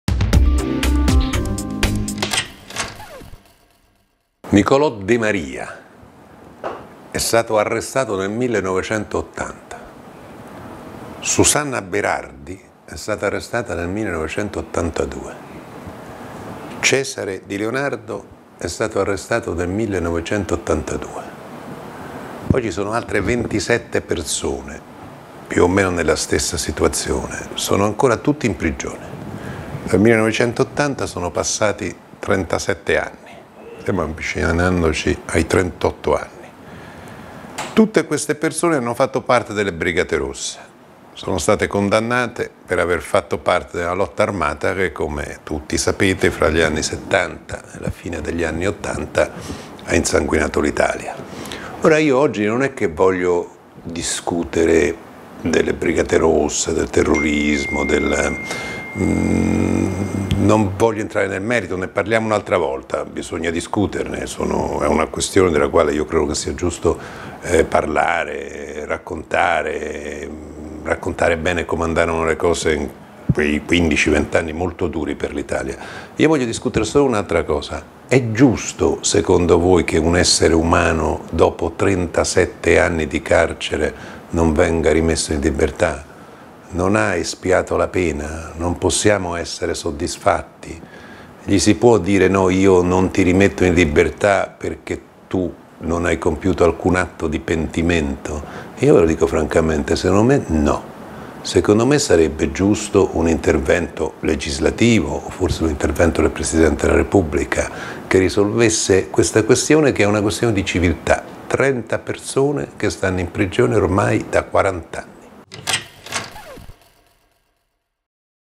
Ci sono 30 ex BR in prigione da più di 30 anni. E' giusto?
Il video editoriale diPiero Sansonetti dell'11/08/2017